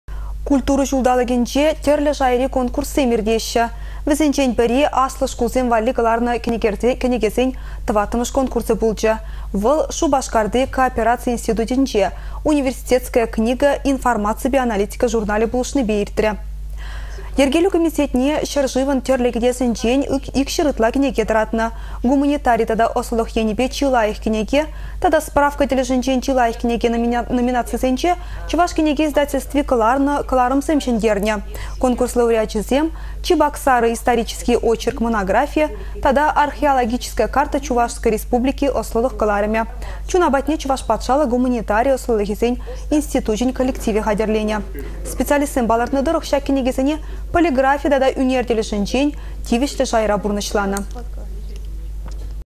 Çĕнĕ кăларăмсем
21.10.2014
Культура çулталăкĕнче тĕрлĕ шайри конкурссем иртеççĕ. Вĕсенчен пĕри-аслă шкулсем валли кăларнă кĕнекесен тăваттăмĕш конкурсĕ. Вăл Шупашкарти коопераци институтĕнче «Университетская книга» информаципе аналитика журналĕ пулăшнипе иртрĕ.